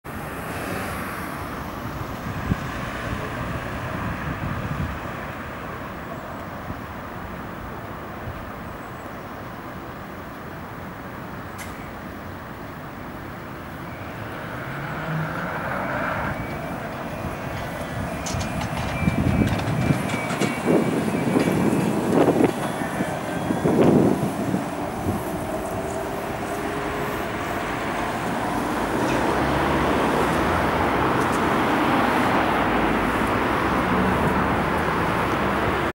Berkhof trolleybus lijn 5 verlaat de Grote Durk en rijd riching zijn eindpunt van Presishaaf
Lijn 5 en 6 spitsen hier bij de halte Grote Durk.
Lijn 5 gaat (zo als je ziet) rechtdoor.
Lijn 6 gaat linksaf richting Eisweide HAN (waar maar 1 tussen halte zit!
Lijn 5 rijd van Presikhaaf via Centraal Station naar Schuytgraaf.